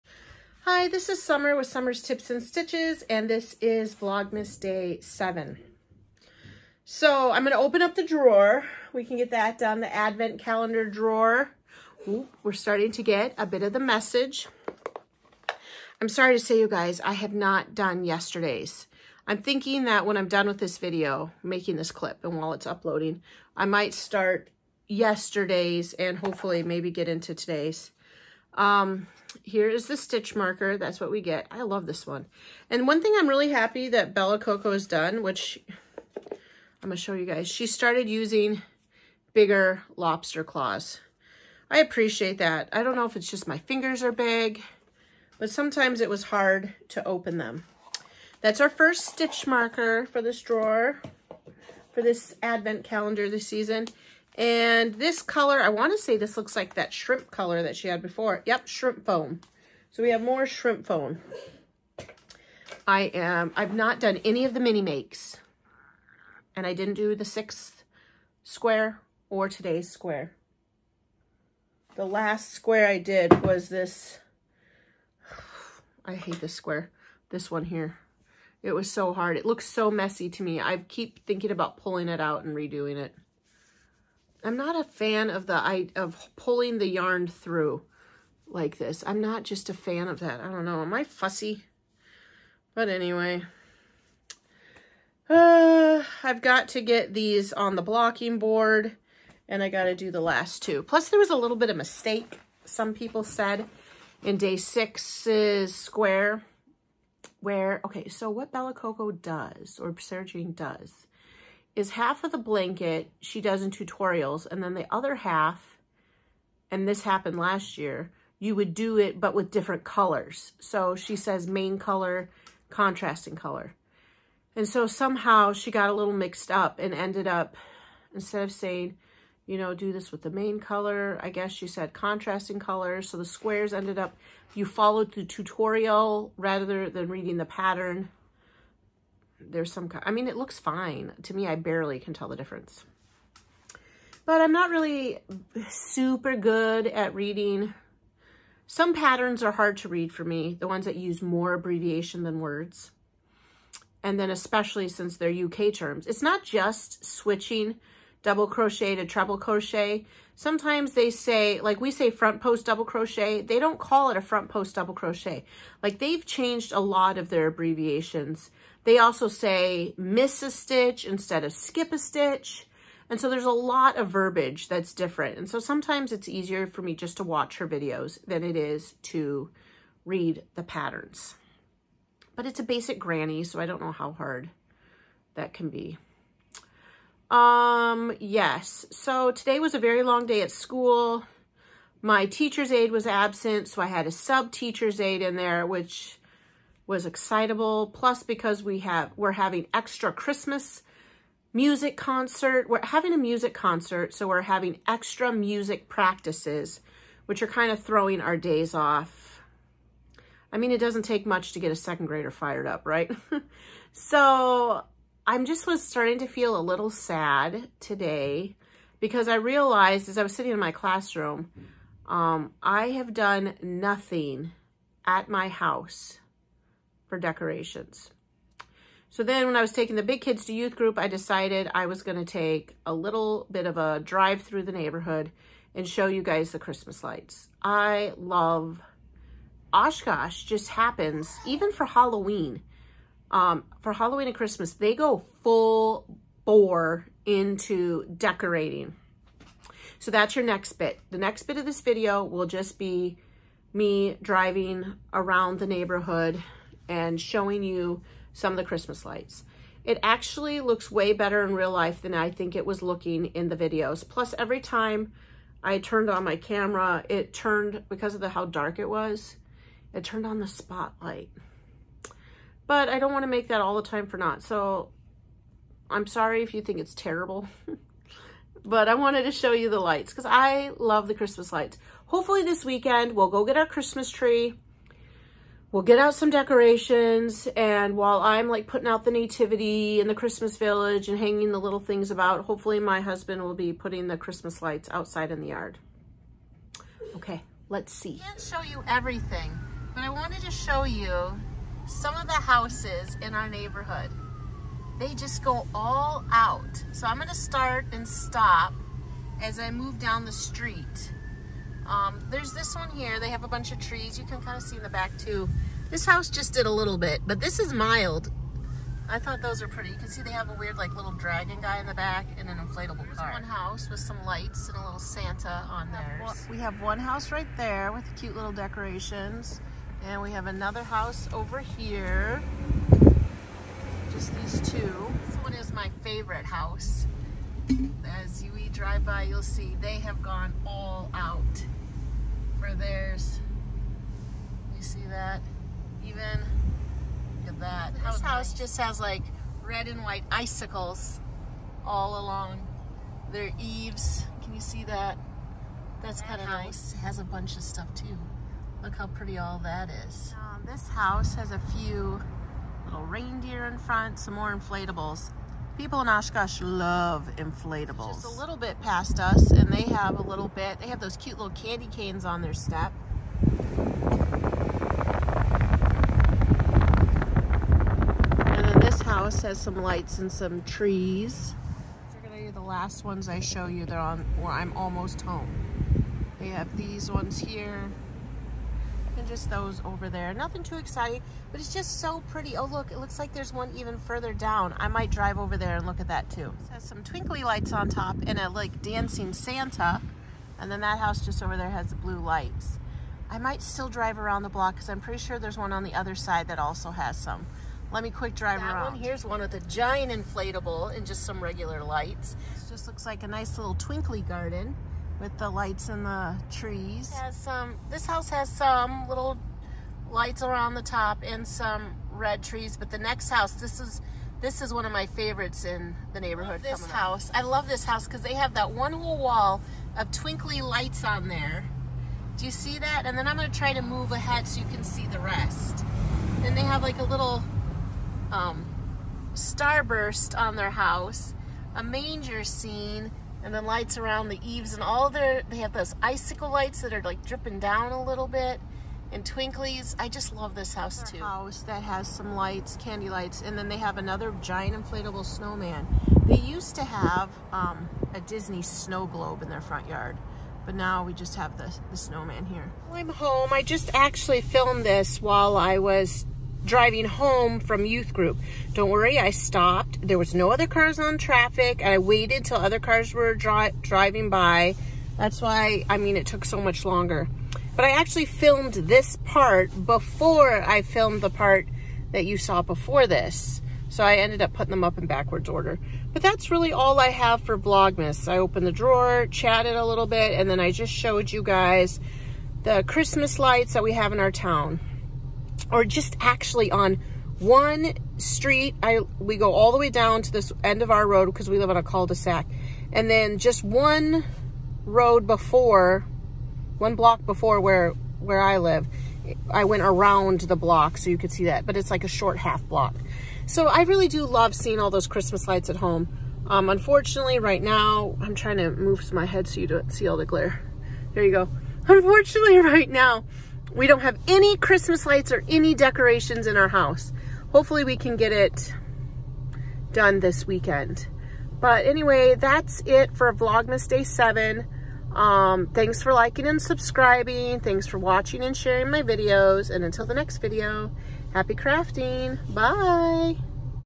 Vlogmas Day 7, 2022
In this video, I open my next advent calendar drawer. My advent calendar comes from crochet Society. I also give you an update on my progress with a Crochet along. And finally after taking the kids to youth group, I show you some of the awesome Christmas lights in my neighborhood.